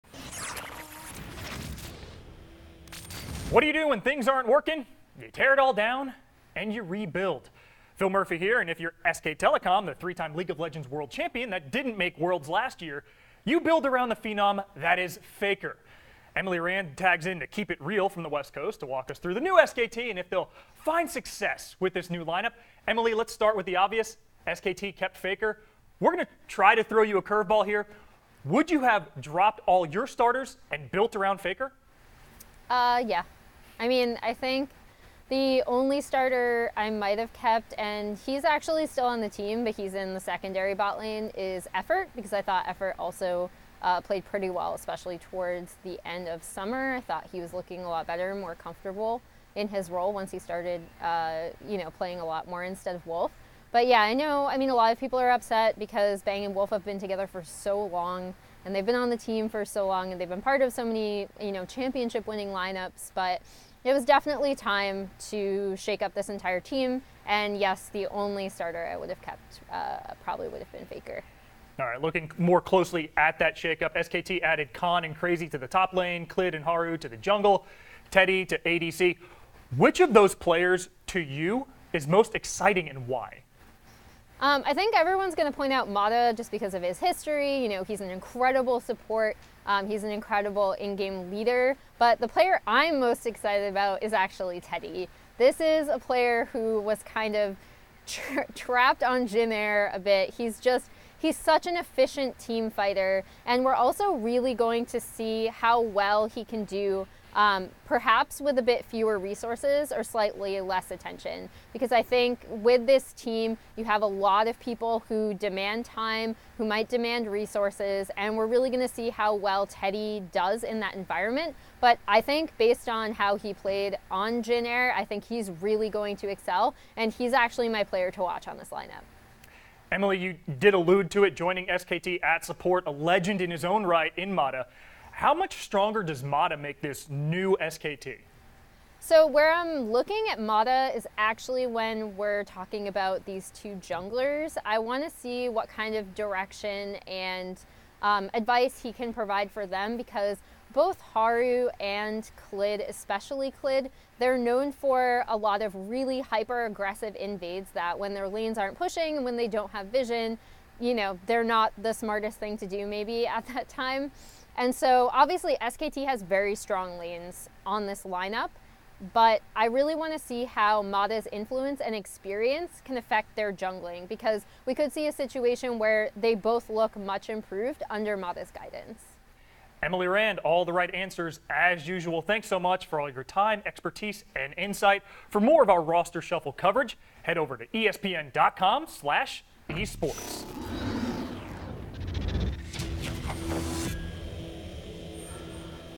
SK Telecom T1 guts roster to rebuild around Faker after an unsuccessful season | ESPN Esports
The South Korean powerhouse SKT T1, known for its dominance in League of Legends has gutted its roster to rebuild around superstar Lee "Faker" Sang-hyeok. Emily Rand breaks down the new roster and explore how the team can work their way back to glory.